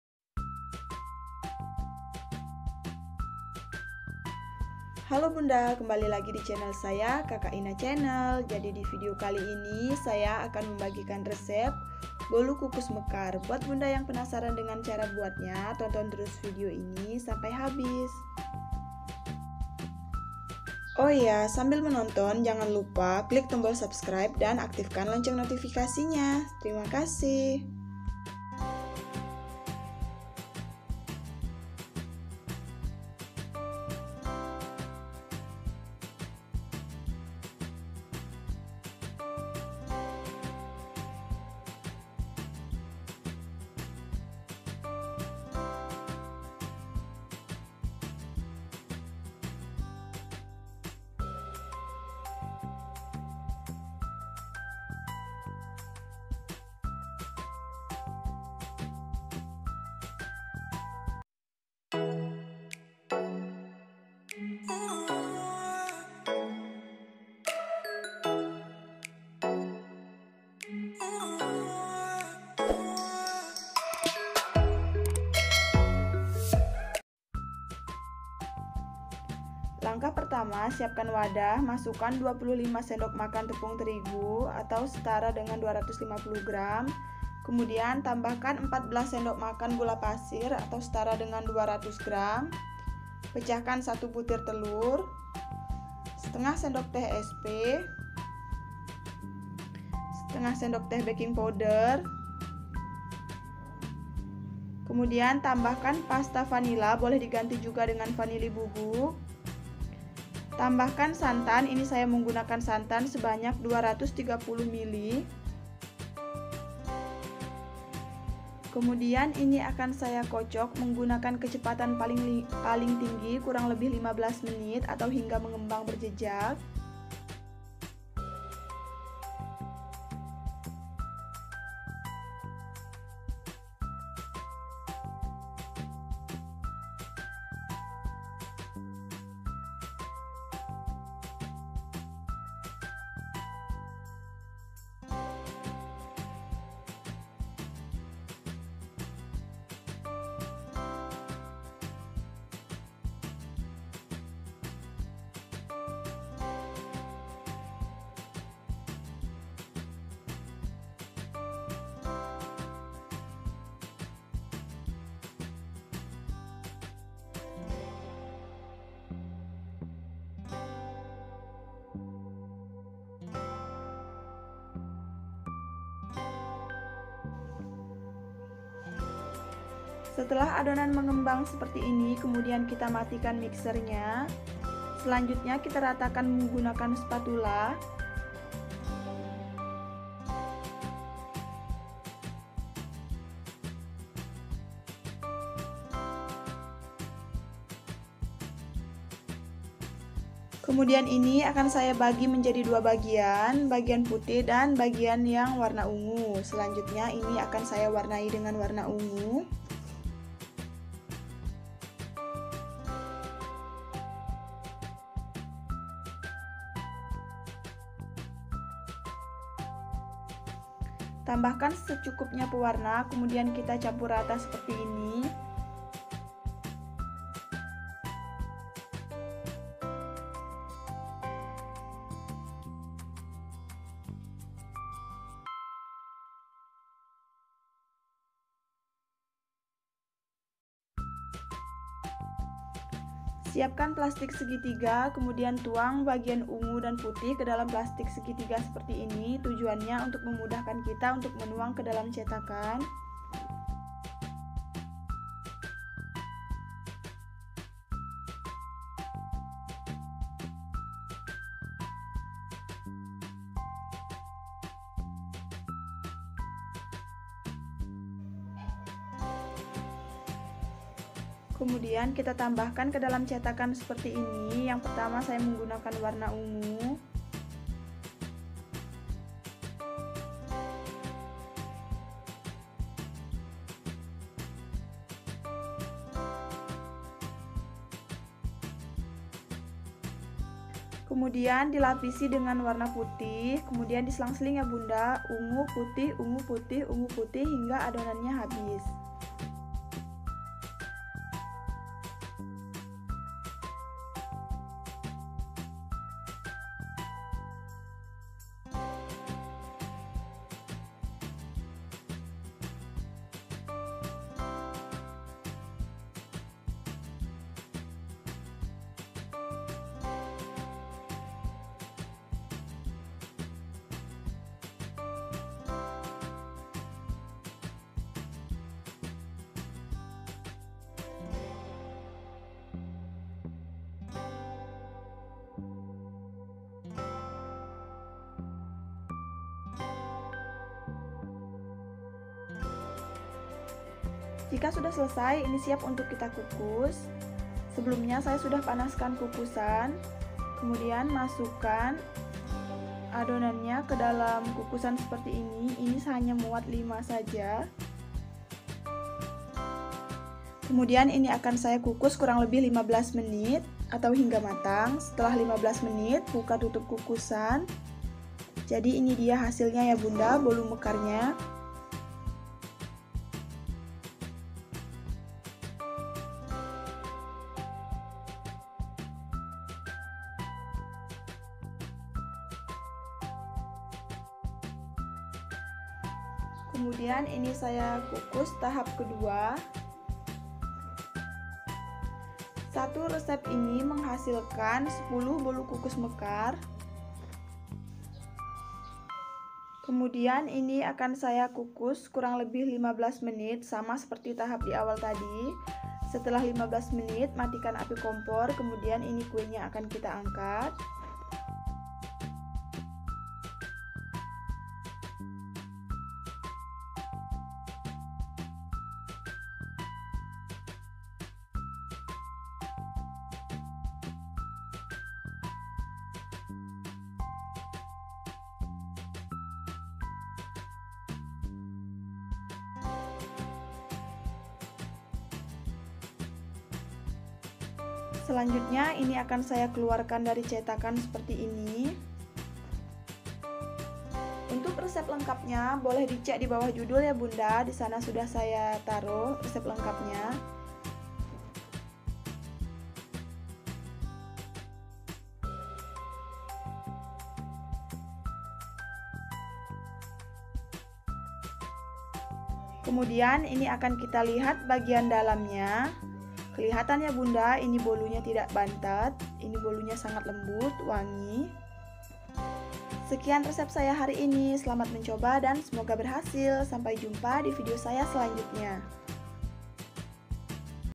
1 telur !!! bolu kukus mekar 1 telur tanpa soda mengembang sempurna
bolu kukus mekar 1 telur

hasilnya mengembang sempurna 😍😍

* 25 sdm terigu (250gr)
* 14 sdm gula (200gr)
* 230 ml santan
* 1/2 sdt Sp
* 1/2 sdt baking powder
* 1 butir telur
* vanilli

tips :
* air kukusan harus benar - benar panas gunakan api besar
* kocok adonan hingga tidak ada udara di dalam adonan